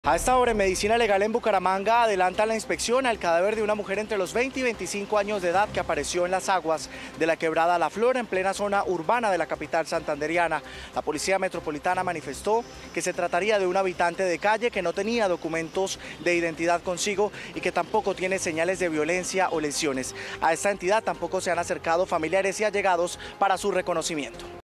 Aparece el cuerpo de una mujer en una quebrada de Bucaramanga
Autoridades creen que se trata de una habitante de calle, de entre 20 y 25 años, que apareció en la quebrada La Flora, en plena zona urbana de Bucaramanga, capital de Santander.

Según los primeros informes, no presentaba señales de violencia.